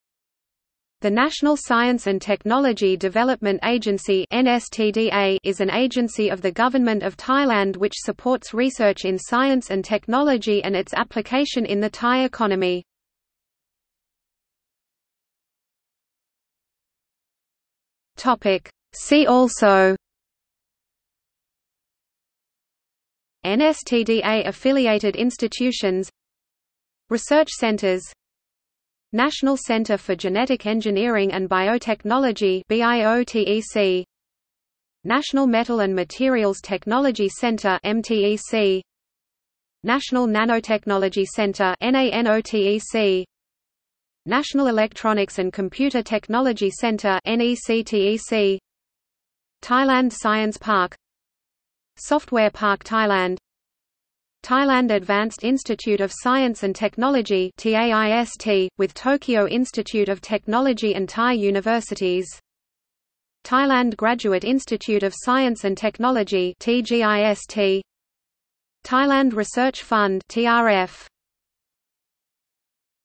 Science and technology in Thailand | Wikipedia audio article
00:00:14 See also

- Socrates

SUMMARY
The National Science and Technology Development Agency (NSTDA) is an agency of the government of Thailand which supports research in science and technology and its application in the Thai economy.